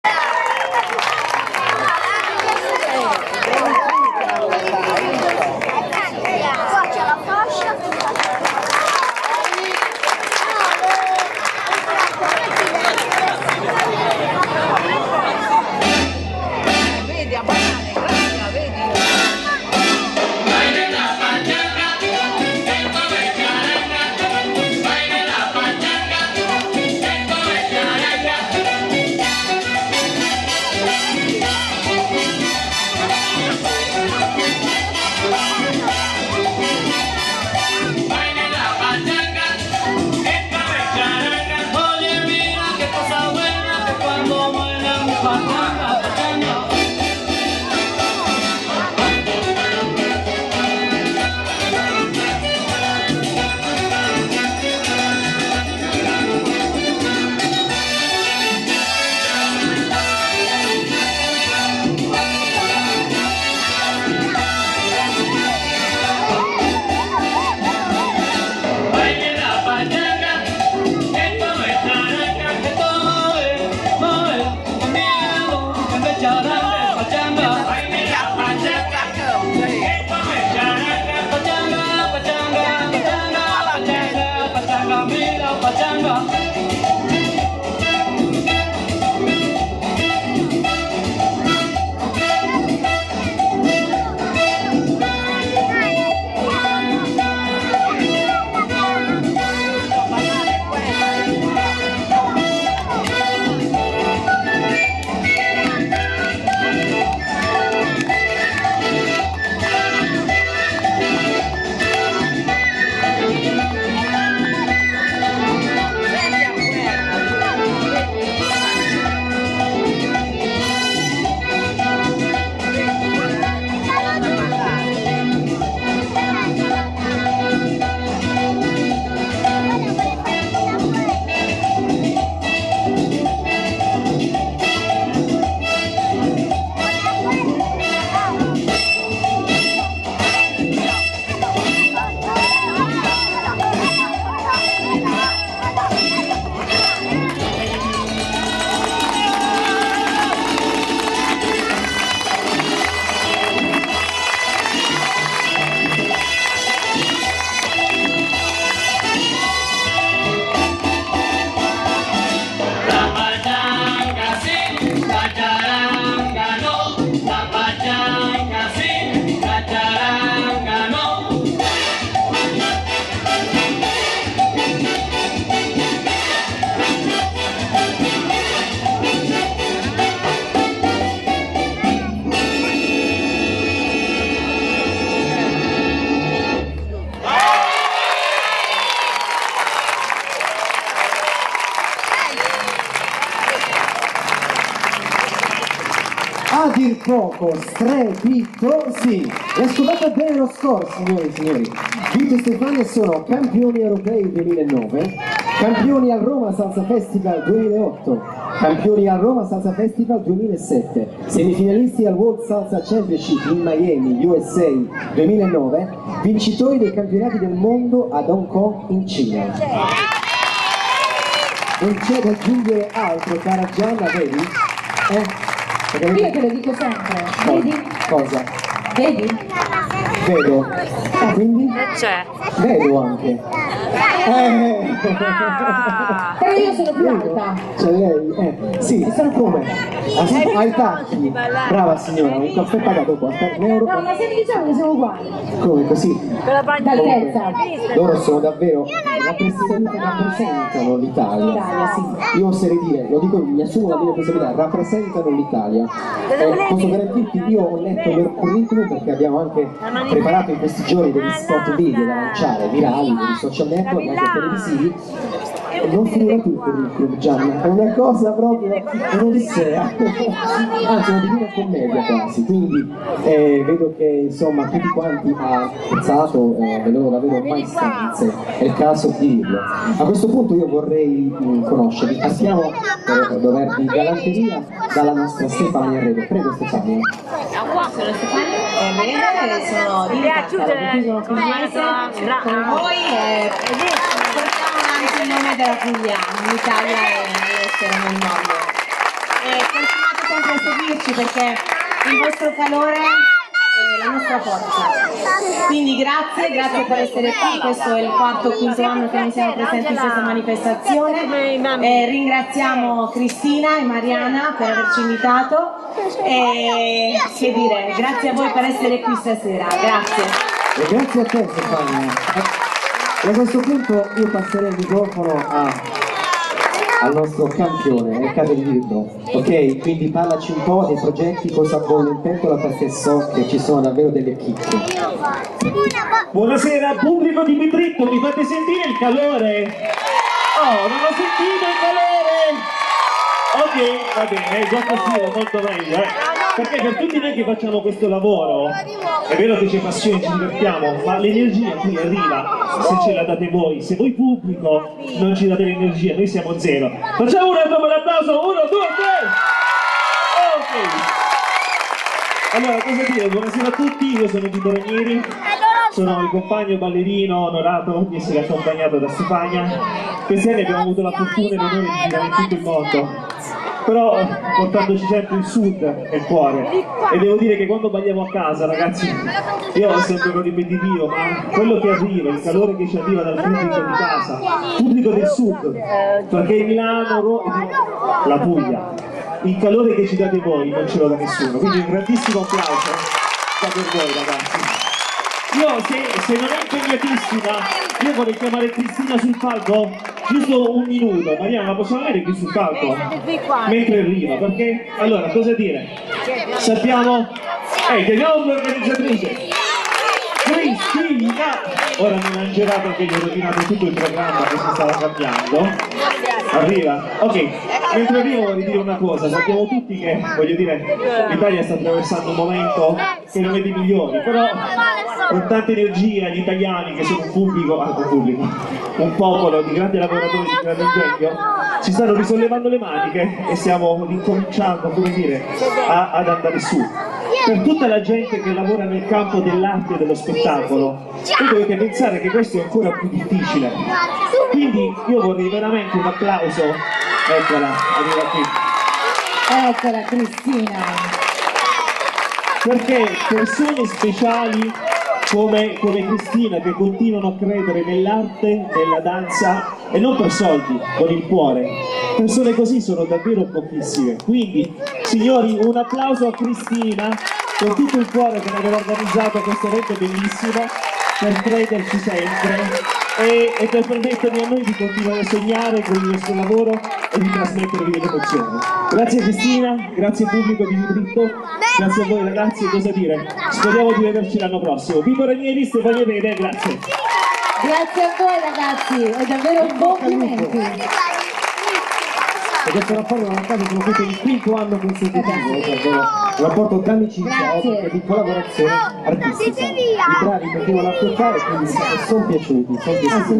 Vito Ranieri e Stefania Errede@ Dance Gala Night show 2014
Vito Ranieri e Stefania Errede ospiti all'evento di Gala "Terra Nostra" nel comune di Bitritto. Vito y Stefania, special guest ormai per 4 anni di fila si esibiscono nel loro nuovo show "Pachanga si, Charanga no", sulle note di Tito Puente. Un ringraziamento alla scuola Tropicalismo Dance di Cristina de Sousa.